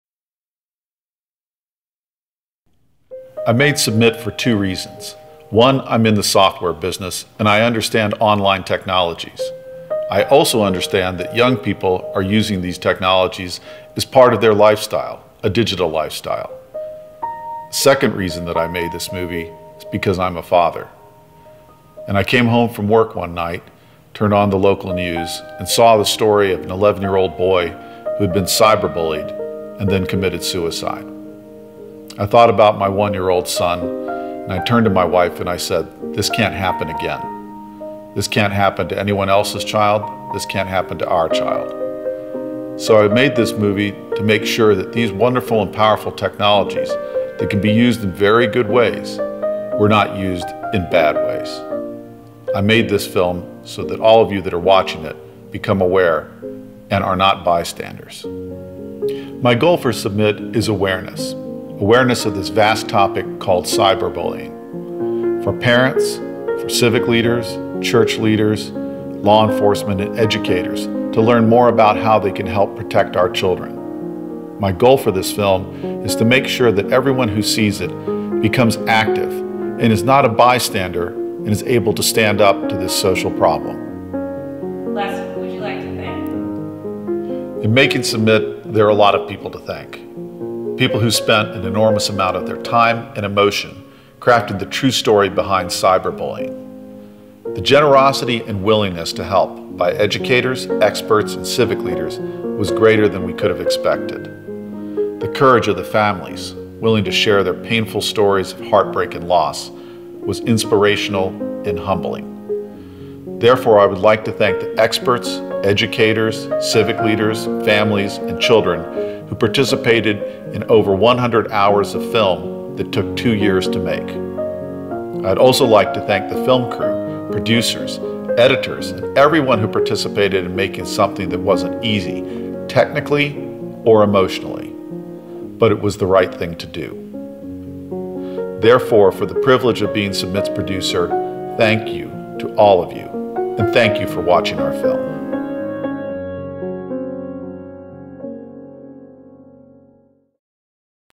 Official Producer's Statement: Les Ottolenghi
Submit compassionately tells the stories of children and families who are affected by the harsh reality of cyberbullying. The shared experiences, testimonials of survivors, and experts on this phenomenon allow you to peer into the rapidly growing epidemic world of cyberbullying. Submit educates parents, teachers, counselors, and bystanders and how they can help the victims of bullying and stop cyberbullies.